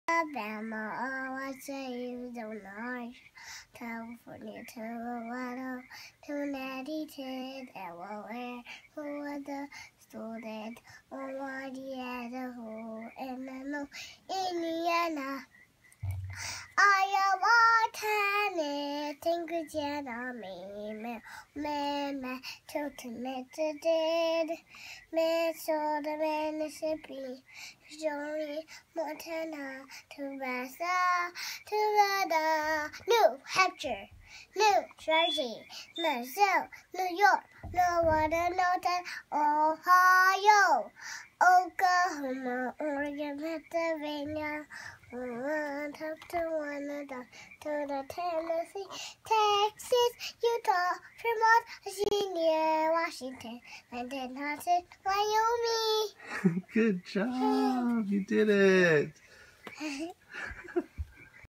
3 year old memorized all 50 states in alphabetical order! Amazing!!
Zia was born on November 15, 2012. She's one smart little girl!